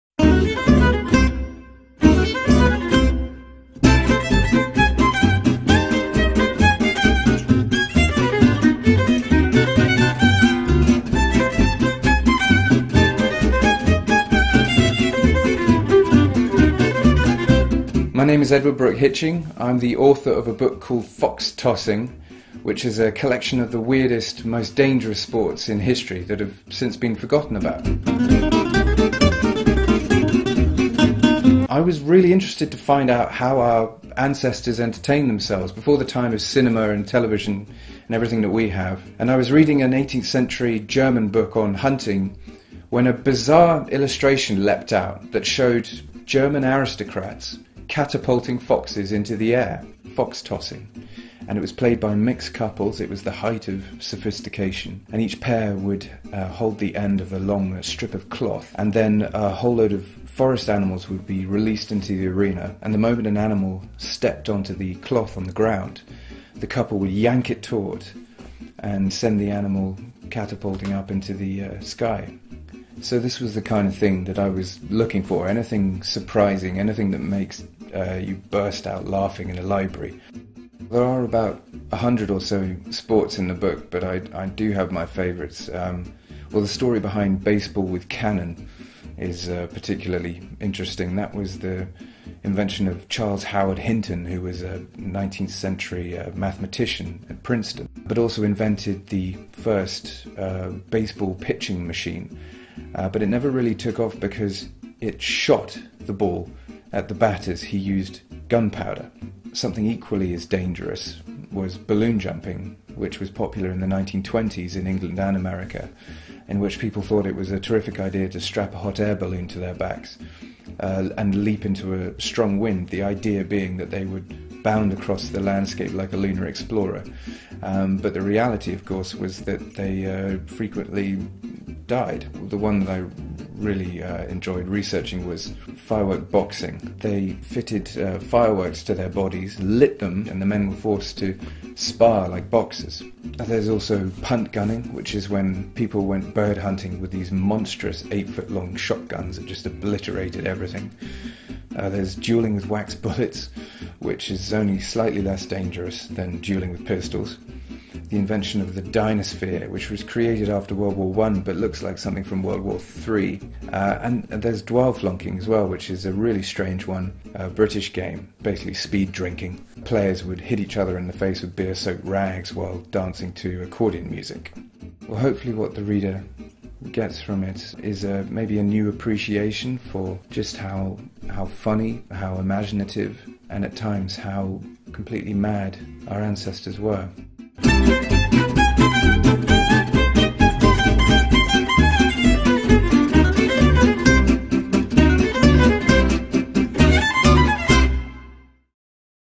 Fox Tossing, Firework Boxing, and Other Forgotten Sports
From octopus wrestling to flagpole sitting, documentary filmmaker and author Edward Brooke-Hitching discusses his fascination with the hilarious, absurd, and downright dangerous sports and pastimes of days gone by, as detailed in his book 'Fox Tossing
And Other Forgotten and Dangerous Sports, Pastimes, and Games'.